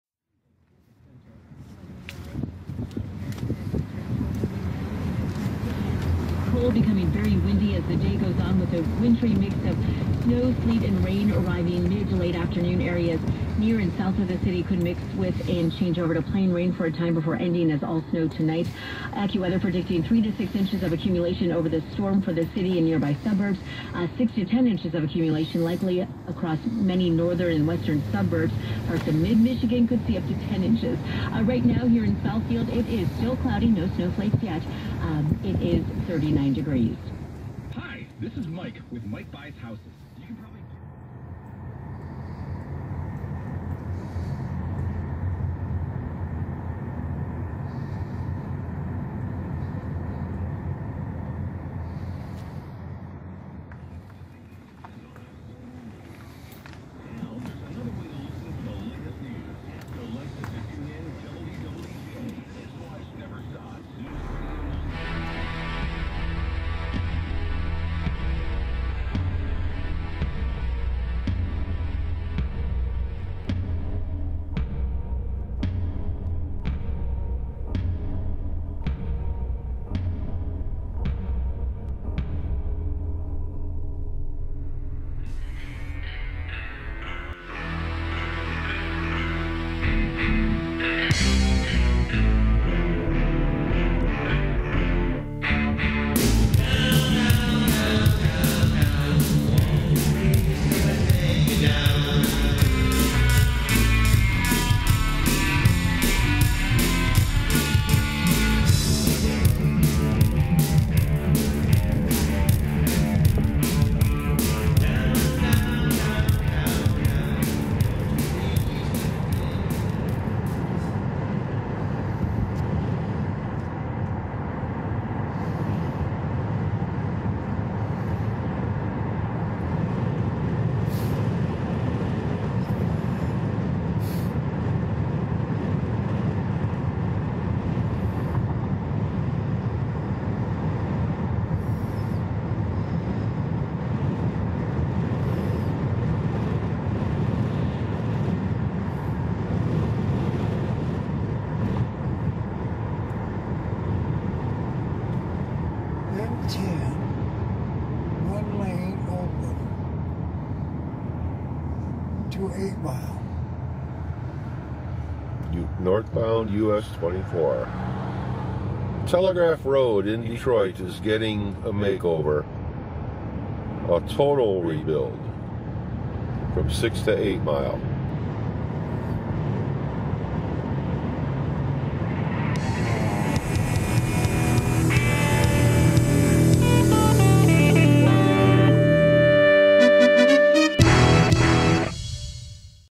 Detroit Inner City Improvements 2023: Brightmoor Demolitions/Telegraph (US-24) Rebuild. Update Zero.
Yellow "WILL DEMOLISH" signs in Brightmoor, @DetroitCameraCrusaders . And a look at Telegraph, which will be totally rebuilt from Grand River to 8 Mile, a 2 year project.